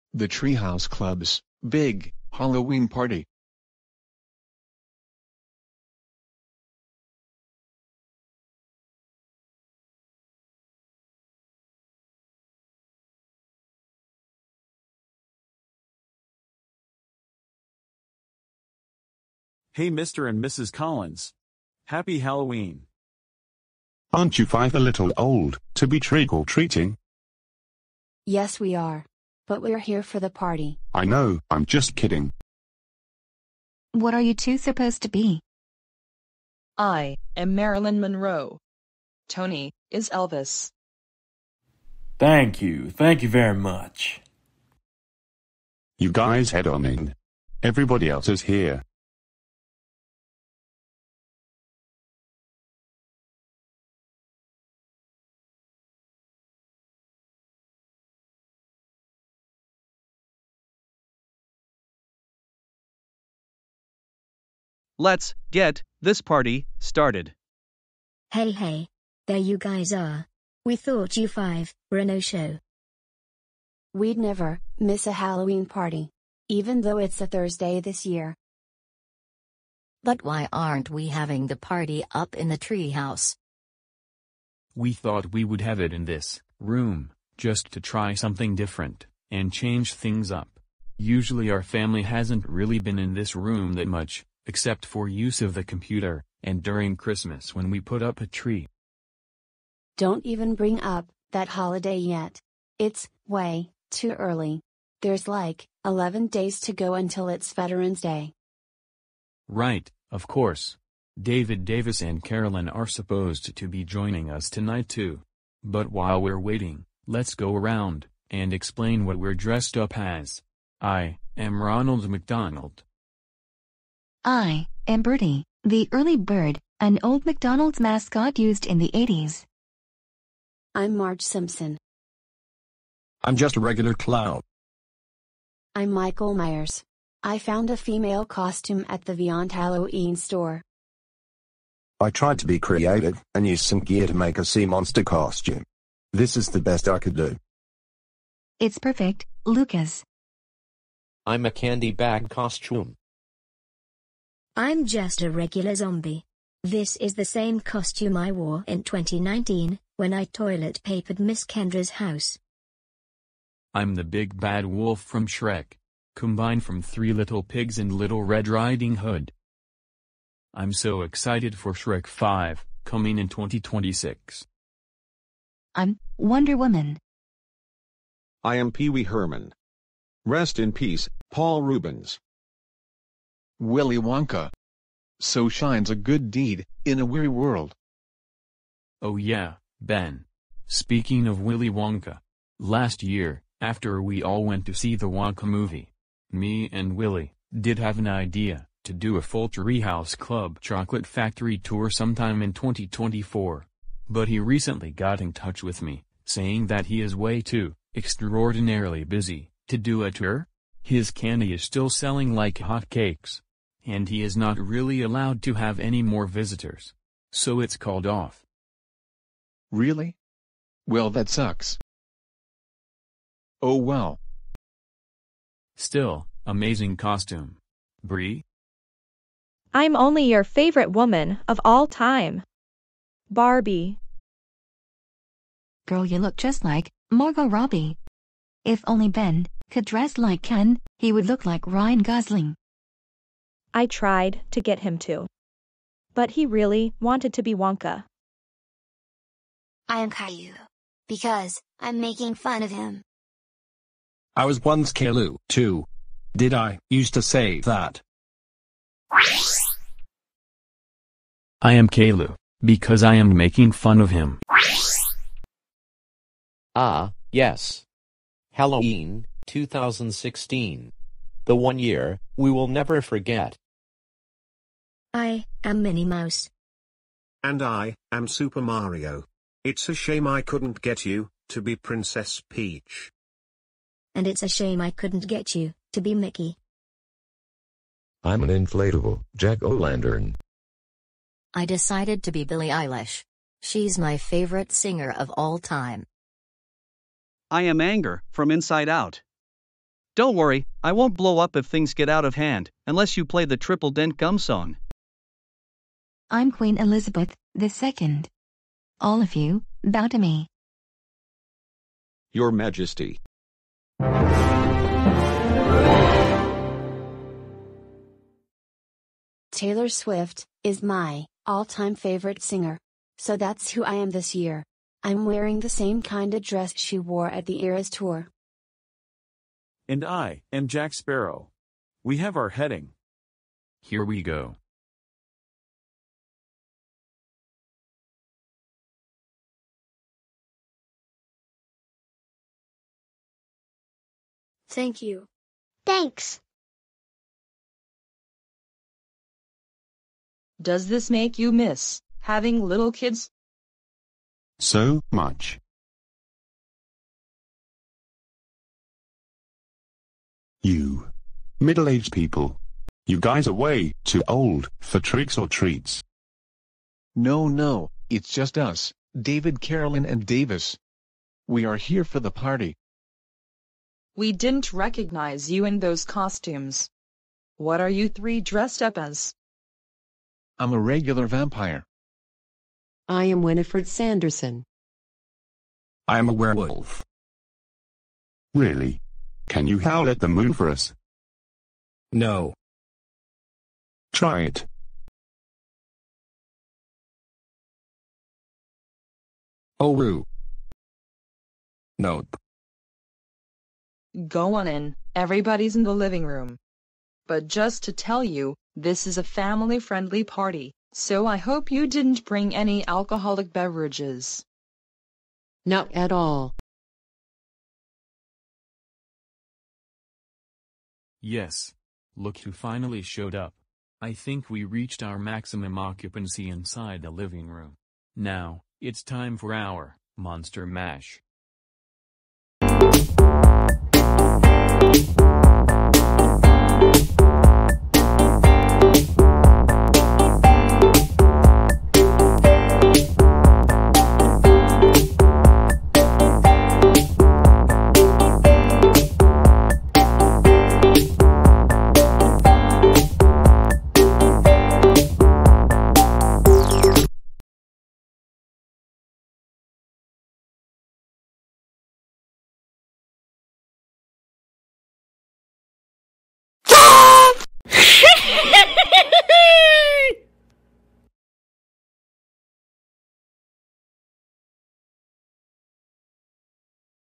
The Treehouse Club's BIG Halloween Party
The Club gathers all together for All Hallows Eve at the Collins House!!!